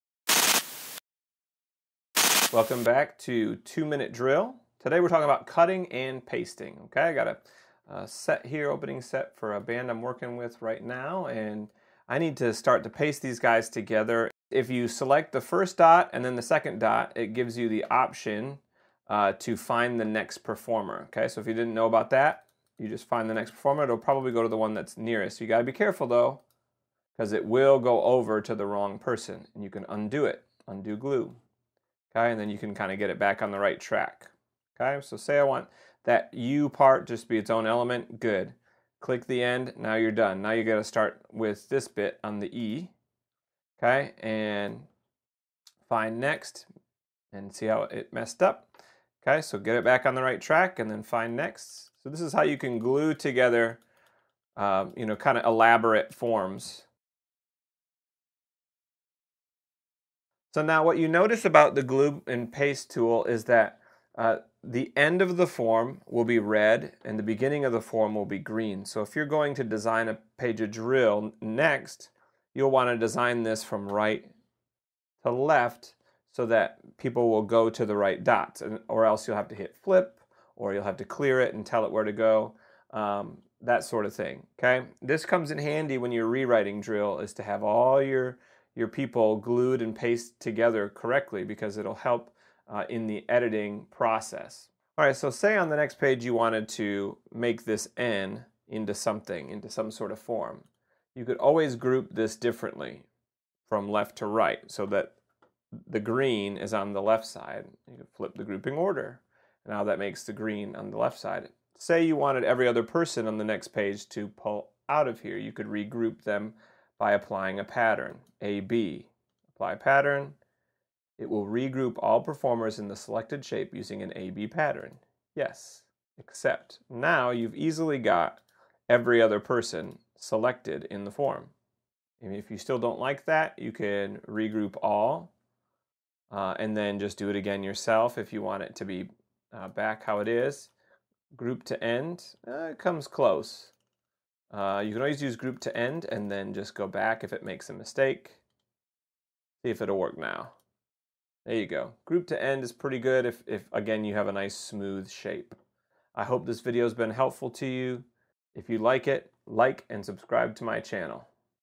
2-Min Drill - Pyware Tutorials - Glue and Paste
These Quick Tutorials are around 2 minutes'ish and cover quick topics and are usually fueled by feedback.  This tutorial has to do with how to Glue and Paste Forms

If you are new to Pyware and would like to take my tutorial I will take you through the basics of how to use the software and my method for writing drill.